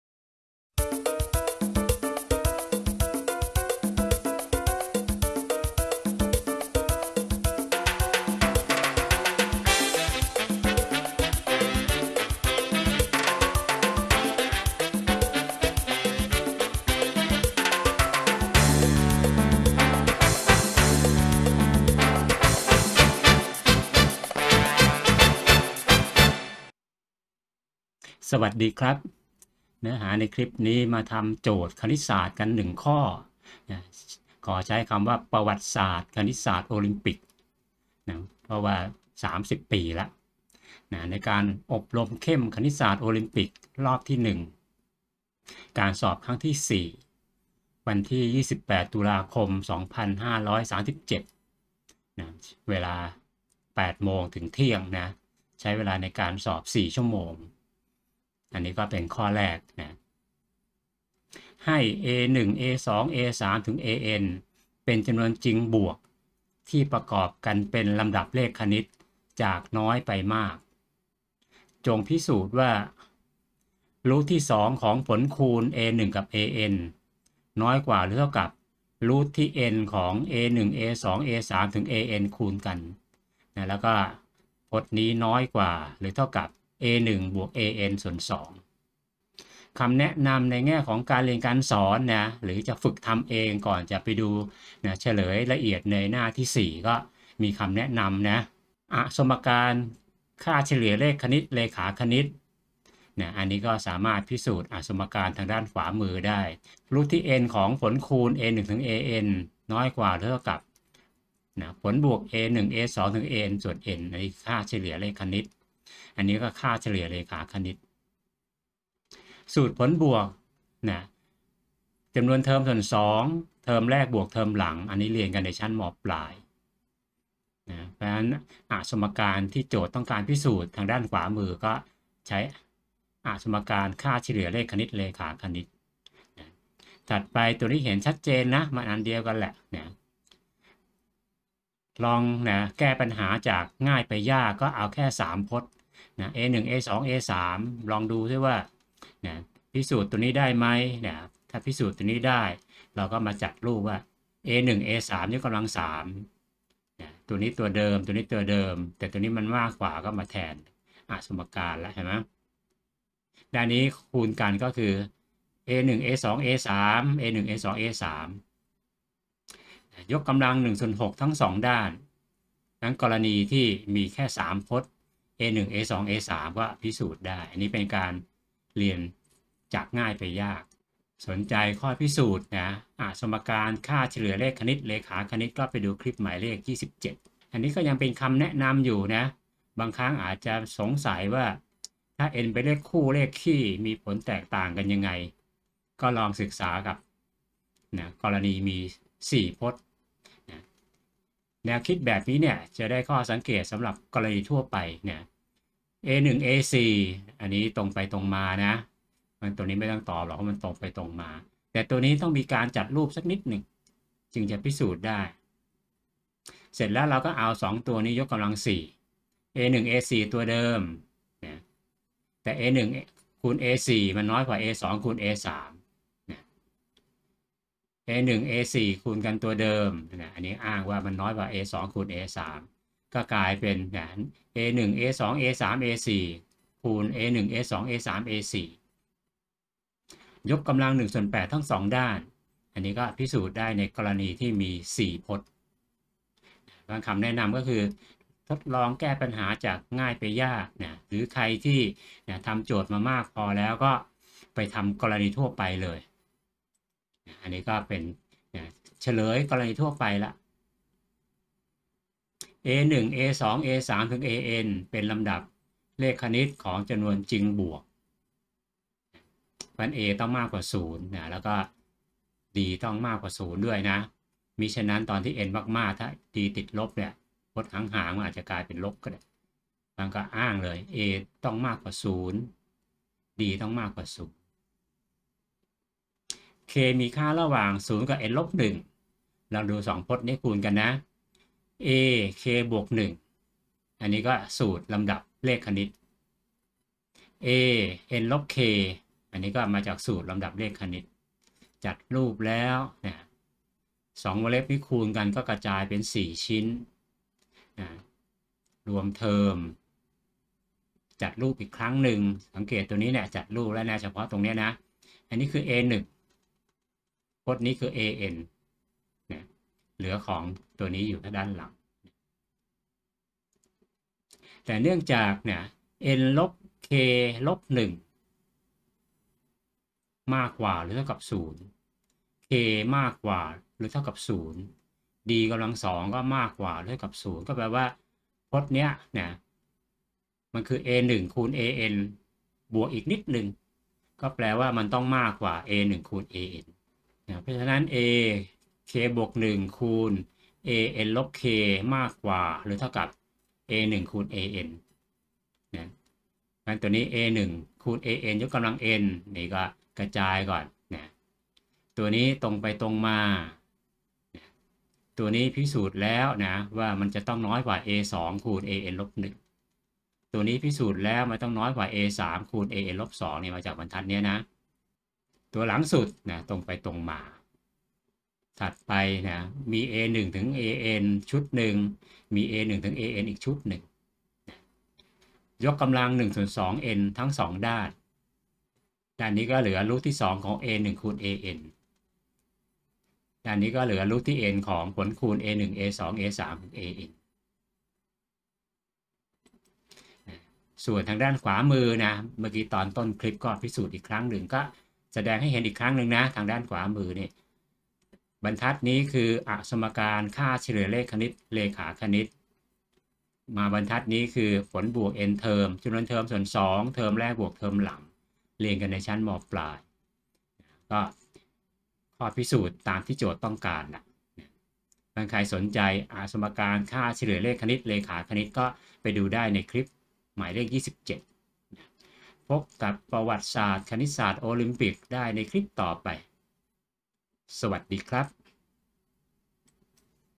Clip 1228 : 01 IMSO History ประวัติศาสตร์คณิตศาสตร์โอลิมปิก
ประวัติศาสตร์คณิตศาสตร์โอลิมปิก
การอบรมเข้มคณิตศาสตร์โอลิมปิก รอบที่ 1 การสอบครั้งที่ 4
วันที่ 28 ตุลาคม 2537 เวลา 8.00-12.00 น.
ข้อ 1